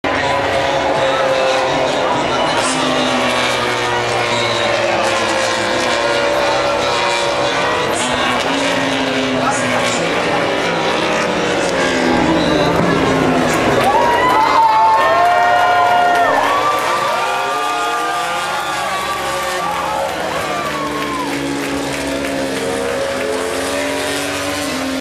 Valentino Rossi Wheelie Misano 2016 Tribuna C
Valentino Rossi doing a Wheelie during the Misano 2016 GP. View from Tribuna C